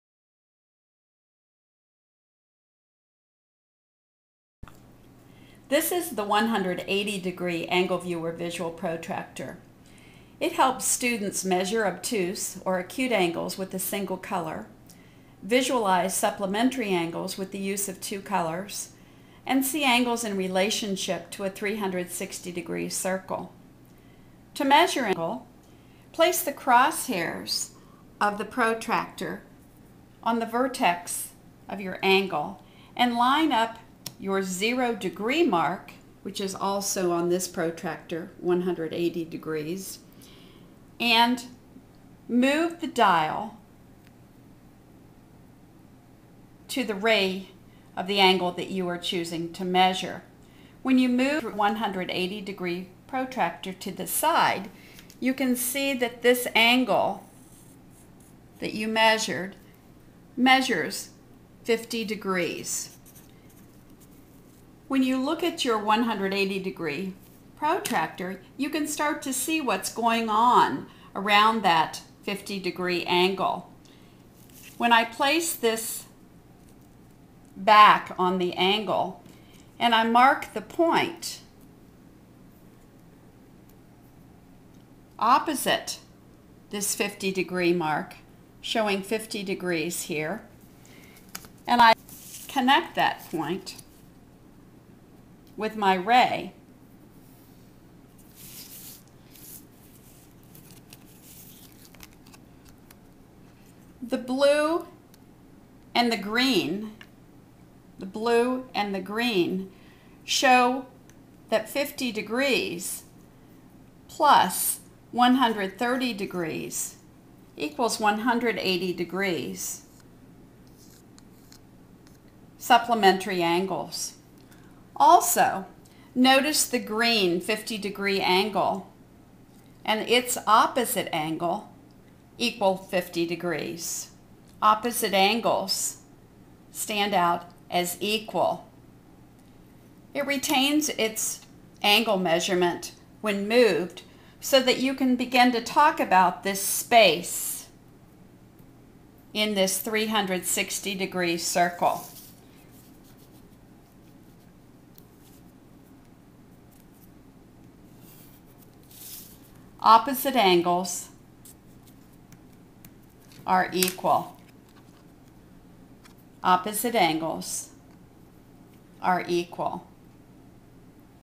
Using the 180 Degree visual protractor (AngleViewer) to understand angles
The 180 Degree AngleViewer can help students:
• Measure obtuse or acute angles with a single color.
• Visualize supplementary angles with the use of two colors.
• Understand reflex angles.
• See angles in relationship to a 360° circle.
In addition:
• Opposite angles stand out as equal.
• Transparent colors enable easy use on an overhead projector.
• Retains angle measurement when moved.